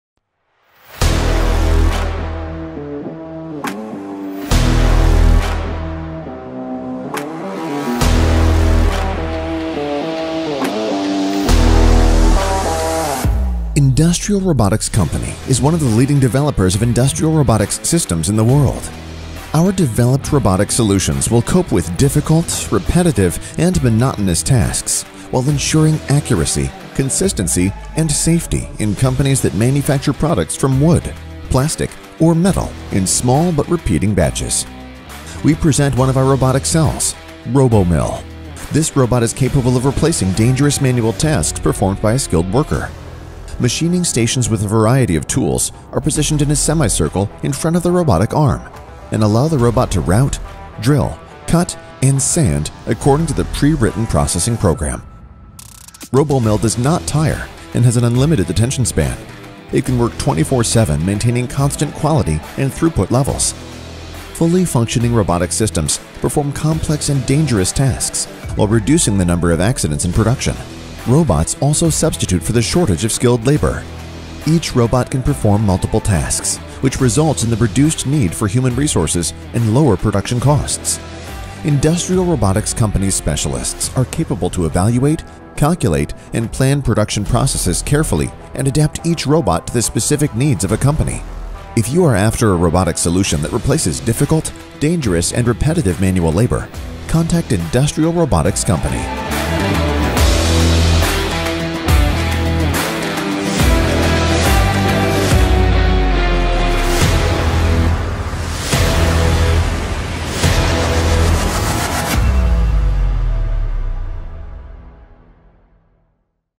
RoboMill – Industrial robot for furniture production (EN subtitles)
RoboMill is an industrial robot - carpenter. Can use different tools and perform different tasks - milling, drilling, cutting, turning, grinding. Adapts to different sizes and shapes of machined components.

The operation of “RoboMill” is based on the ability to use multiple machinery tools simultaneously. Made to operate on various component manufacturing stages, the robot allows proceeding with the help of different tools. “RoboMill” is designed to mill, drill, cut, plan, turn, sand, forge and assemble components accurately.